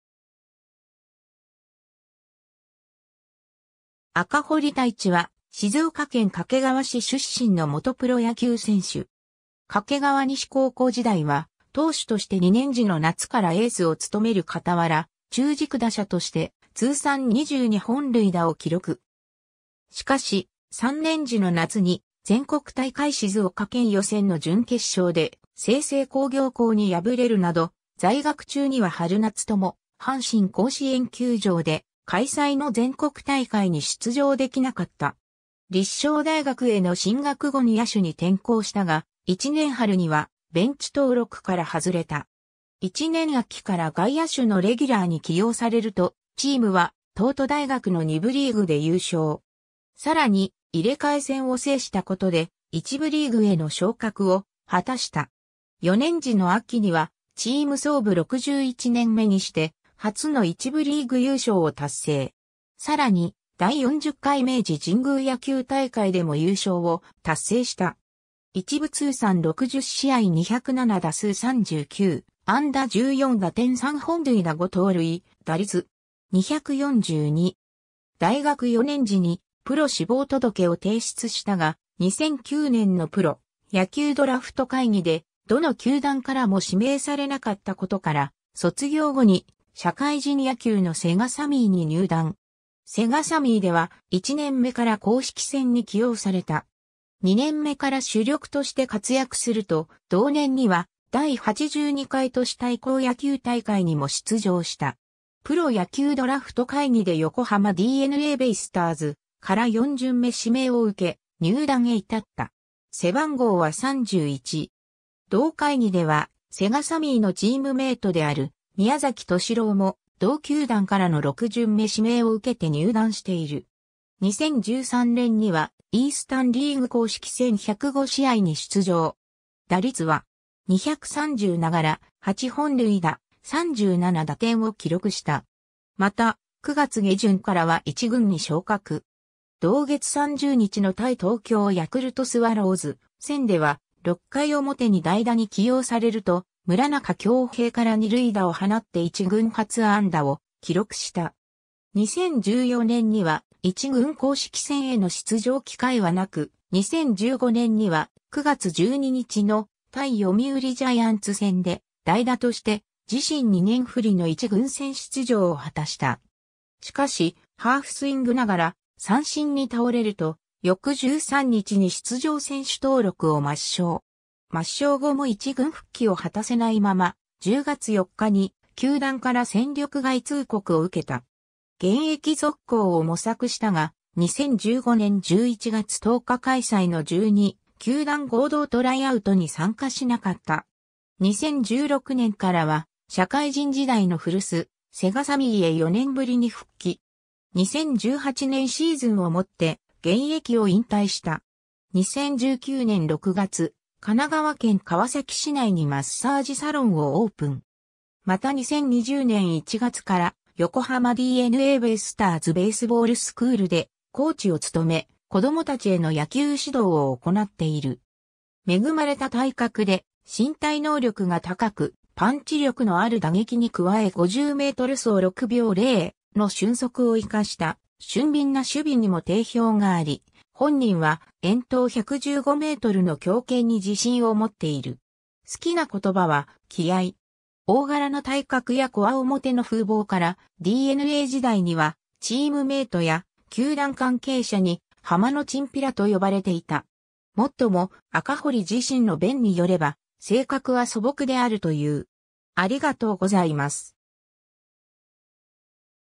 赤堀大智
赤堀 大智（あかほり だいち、1987年4月13日 - ）は、静岡県掛川市出身の元プロ野球選手（外野手）。
 掛川西高校時代は、投手として2年時の夏からエースを務めるかたわら、中軸打者として通算22本塁打を記録。
しかし、3年時の夏に全国大会静岡県予選の準決勝で静清工業高に敗れるなど、在学中には春夏とも阪神甲子園球場で開催の全国大会に出場できなかった。
 立正大学への進学後に野手に転向したが、1年春にはベンチ登録から外れた。
1年秋から外野手のレギュラーに起用されると、チームは東都大学の2部リーグで優勝。
さらに、入れ替え戦を制したことで、1部リーグへの昇格を果たした。
4年時の秋には、チーム創部61年目にして初の1部リーグ優勝を達成。
さらに、第40回明治神宮野球大会でも優勝を達成した。
1部通算60試合207打数39安打14打点3本塁打5盗塁、打率.242。
 大学4年時にプロ志望届を提出したが、2009年のプロ野球ドラフト会議でどの球団からも指名されなかったことから、卒業後に社会人野球のセガサミーに入団。
セガサミーでは、1年目から公式戦に起用された。
2年目から主力として活躍すると、同年には第82回都市対抗野球大会にも出場した。
 プロ野球ドラフト会議で横浜DeNAベイスターズから4巡目指名を受け、入団へ至った。
同会議ではセガサミーのチームメイトである宮﨑敏郎も同球団からの6巡目指名を受けて入団している。
 2013年には、イースタン・リーグ公式戦105試合に出場。
打率は.230ながら、8本塁打、37打点を記録した。
また、9月下旬からは一軍に昇格。
同月30日の対東京ヤクルトスワローズ戦（神宮球場）では、6回表に代打に起用されると、村中恭兵から二塁打を放って一軍初安打を記録した。
 2014年には、一軍公式戦への出場機会はなく、2015年には、9月12日の対読売ジャイアンツ戦（東京ドーム）で、代打として自身2年振りの一軍戦出場を果たした。
しかし、ハーフスイングながら三振に倒れると、翌13日に出場選手登録を抹消。
抹消後も一軍復帰を果たせないまま、10月4日に球団から戦力外通告を受けた。
 現役続行を模索したが、2015年11月10日開催の12球団合同トライアウト（草薙球場）に参加しなかった。
 2016年からは、社会人時代の古巣・セガサミーへ4年ぶりに復帰。
2018年シーズンをもって現役を引退した。
 2019年6月、神奈川県川崎市内にマッサージサロンをオープン。
また2020年1月から横浜DeNAベイスターズベースボールスクールでコーチを務め、子供達への野球指導を行っている。
 恵まれた体格で身体能力が高く、パンチ力のある打撃に加え50メートル走6秒0の俊足を活かした俊敏な守備にも定評があり、本人は遠投115メートルの強肩に自信を持っている。
 好きな言葉は「気合」。
大柄な体格や強面の風貌から、DeNA時代には、チームメイトや球団関係者にハマのチンピラと呼ばれていた。
もっとも、赤堀自身の弁によれば、性格は素朴であるという。